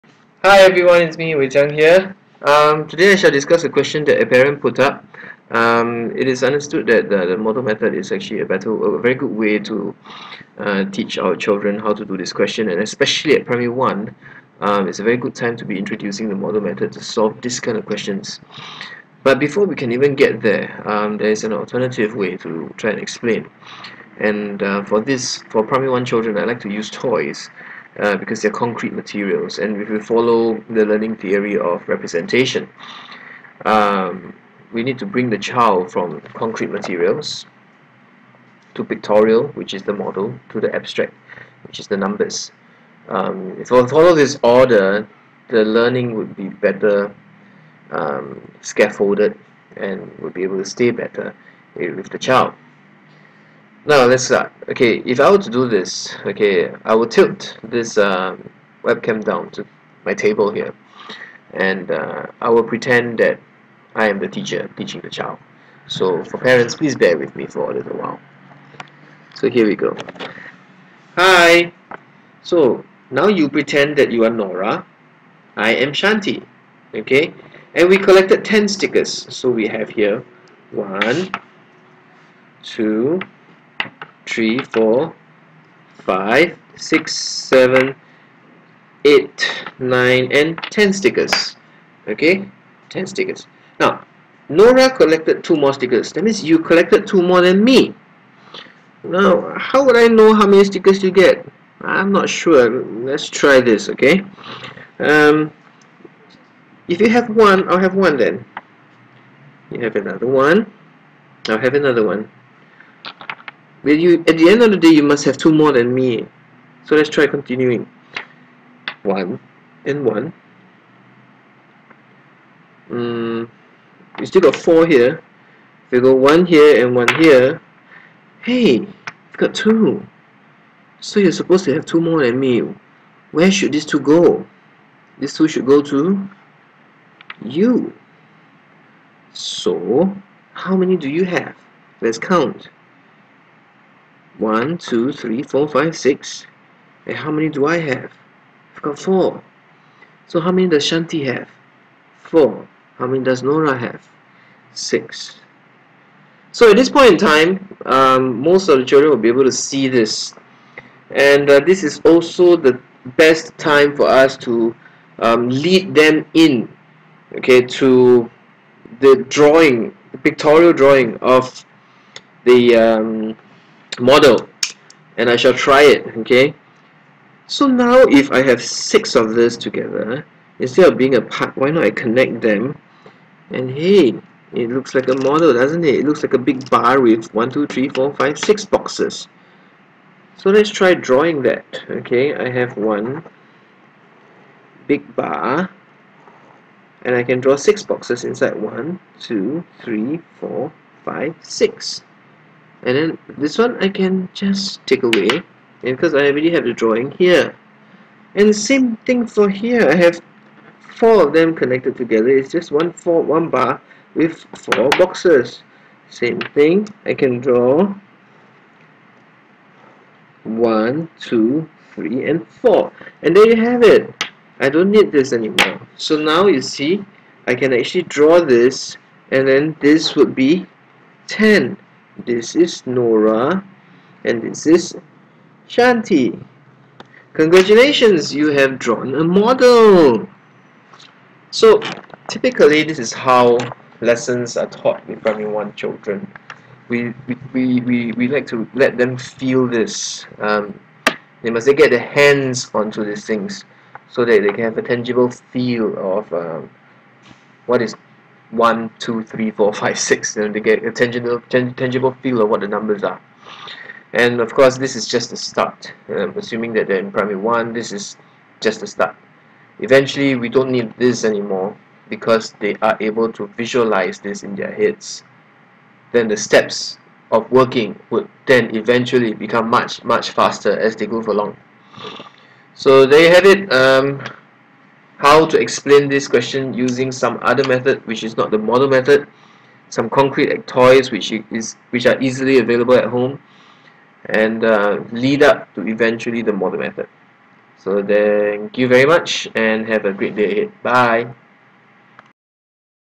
Primary 1 Math - using toys to lead to model drawing.
Today I shall discuss a question that was posed to a Primary 1 question and I will demonstrate how to teach it to a Primary 1 child. I will make use of the CPA approach and show how to make use of everyday toys to lead a child into drawing models and eventually understanding how the model method works. Hope this video helps!

Drop me an email to discuss how I can help your child with Math!